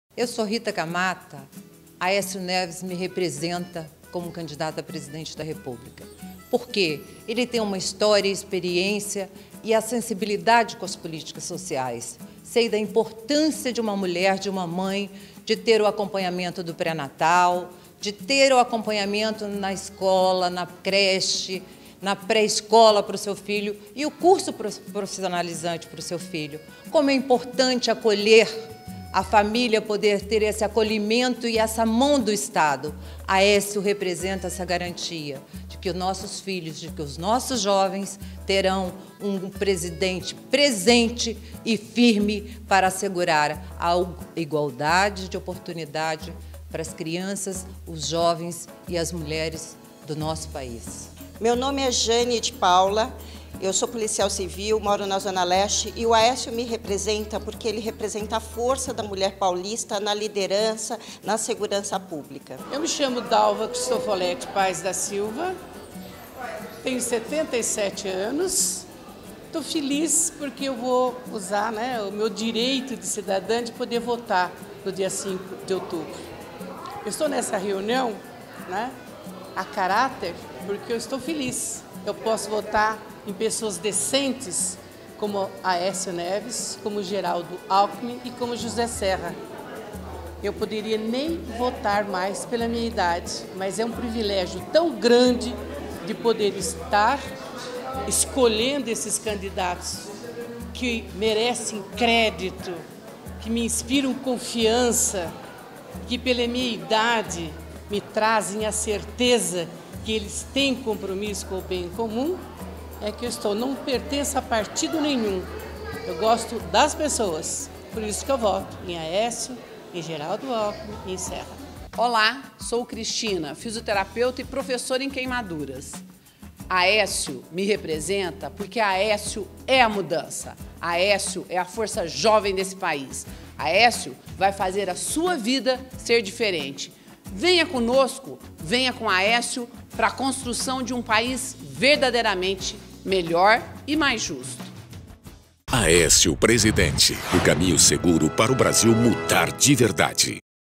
Aécio Neves - BEM-VINDO À UNIÃO
As mulheres que são guerreiras votam em Aécio Neves porque ele representa todos os brasileiros que, apesar de terem um governo federal ineficiente, são capazes de superar as dificuldades e lutar por país melhor.